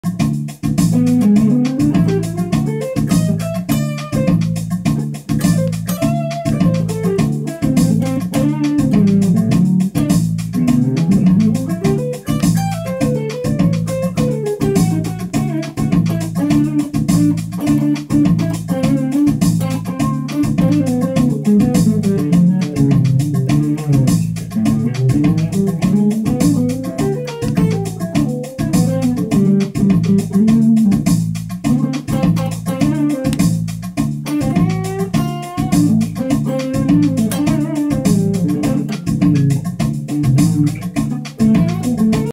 Peter Fischer - short jam over D7, part 1
Me playing a few Pat Martino inspired lines over D7.